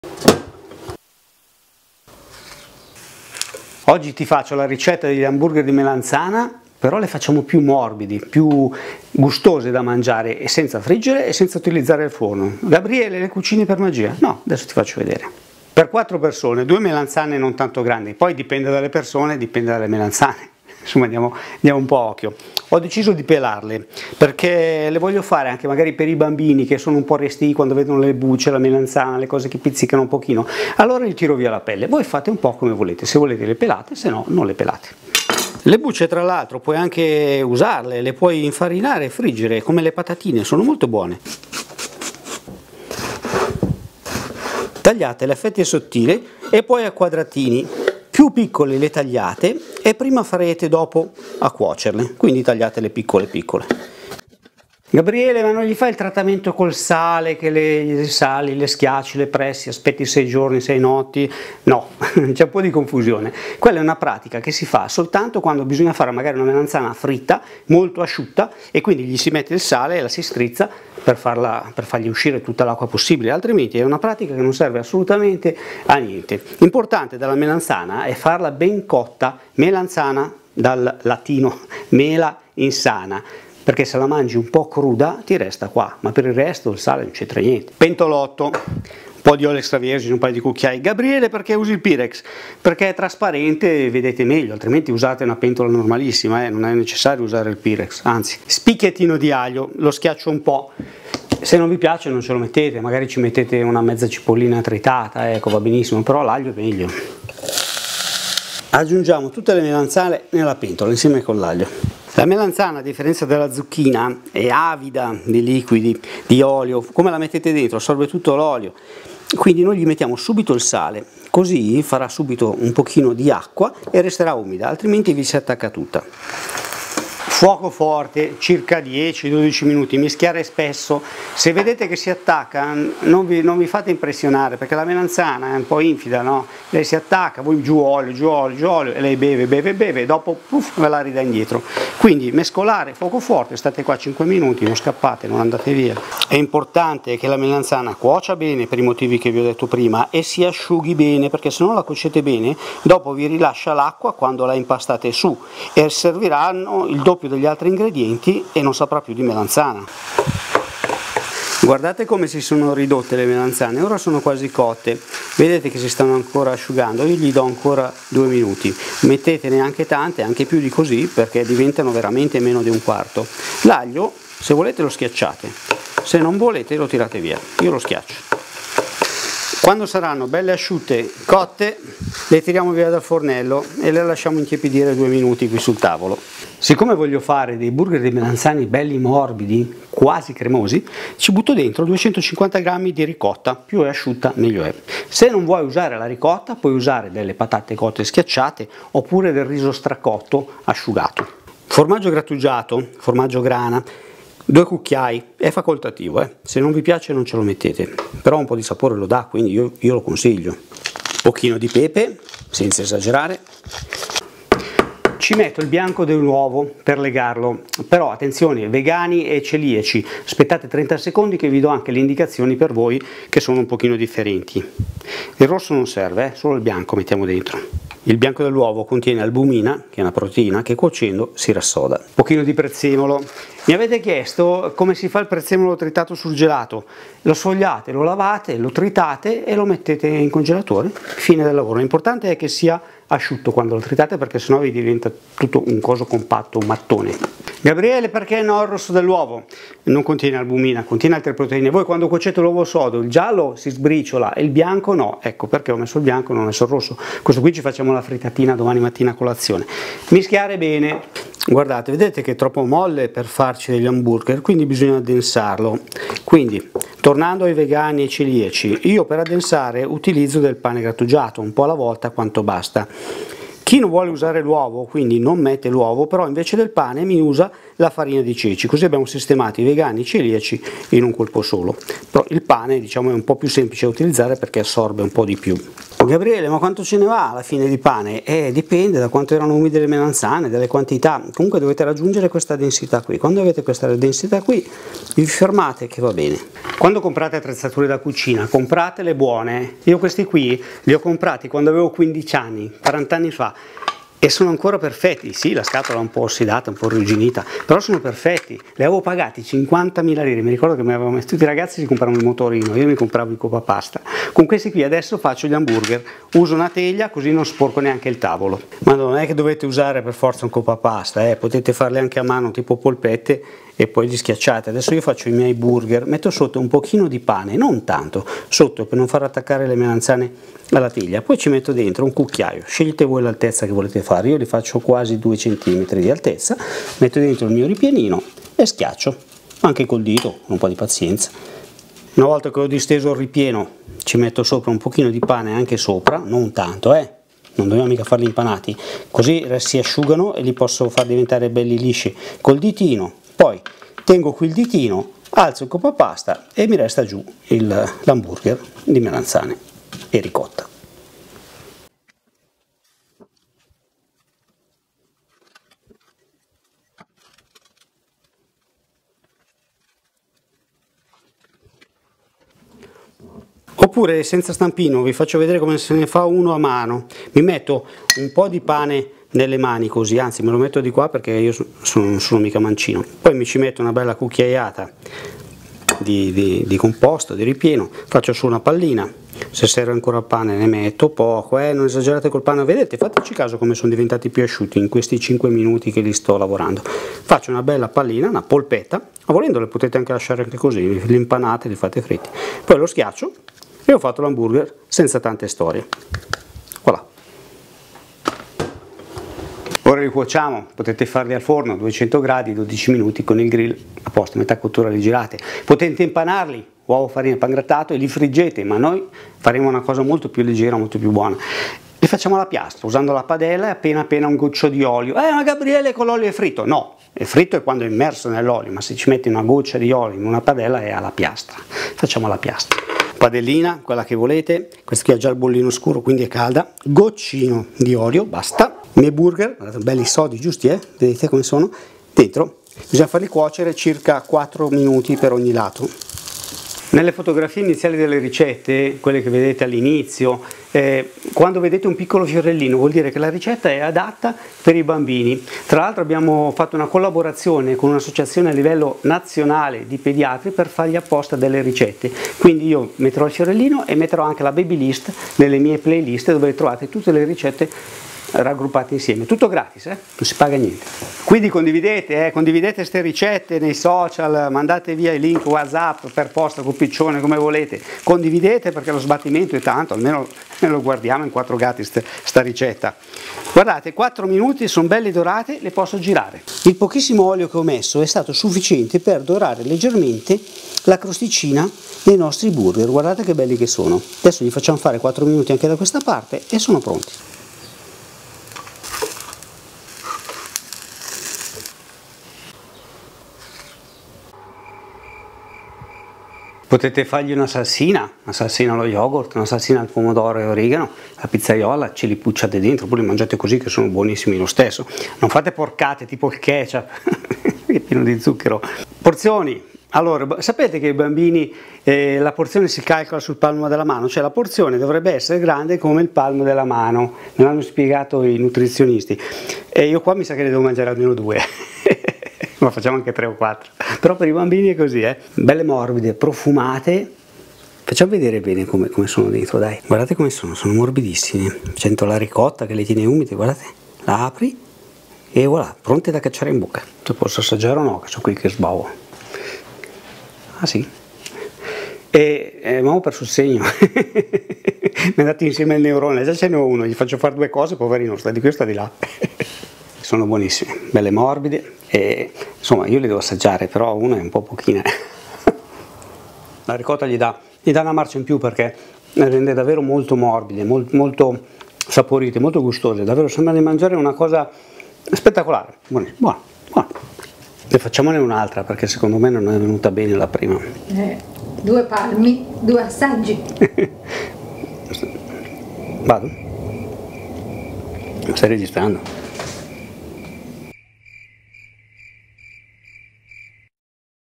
BURGER DI MELANZANE e RICOTTA SENZA FRIGGERE SENZA FORNO burger di melanzane e ricotta
Burger di melanzane e ricotta
QUI TROVI LE COSE CHE USO
ricette rapanello
Senza friggere e senza forno

2 melanzane medie
Aglio uno spicchio
Ricotta 250 gr
Un Albume
Formaggio 2 cucchiai
sale pepe prezzemolo
Pane qb

Lavare, pelare e tagliare a piccoli cubi le melanzane.
In una casseruola metti 2 cucchiai di olio Extravergine e uno spicchio di aglio a rosolare, e aggiungi le melanzane.
Subito il sale.
Cuocere mischiando spesso a fuoco medio per circa 10-12 minuti.
Le melanzane si ridurranno molto e cuoceranno asciugando la loro acqua.
Togliere sale fornello, aggiungi la ricotta, pepe, prezzemolo, 2 cucchiai di formaggio e mischiare, aggiungi l'albume di un uovo e pane grattugiato per addensare.
Chi segue una dieta vegana e i celiaci possono omettere l'albume e utilizzare direttamente della farina di ceci per addensare.
Ottenuto un composto più asciutto potete creare delle polpette e poi schiacciarle per creare i burger.
Oppure utilizzate uno stampino per formare i burger di melanzane.
Le mani dovranno essere unte, bagnate, oppure infarinate con del pane.
Questi burger possono essere panati in uovo e pane e fritti, oppure cotti così come sono nel forno con il grill acceso, oppure, come in questo caso, basterà una padella leggermente unta, e cotti direttamente come fosse una piastra, 4 minuti per ogni lato.